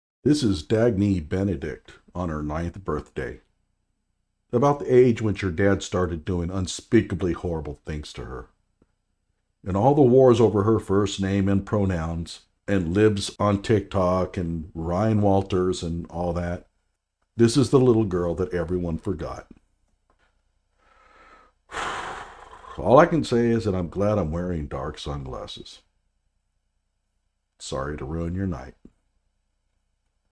Dagny "Nex" Benedict, Age 9
Maybe we have all forgotten the most important thing about the Nex Benedict tragedy.

I thiank iMayBKGB for the source FB photo.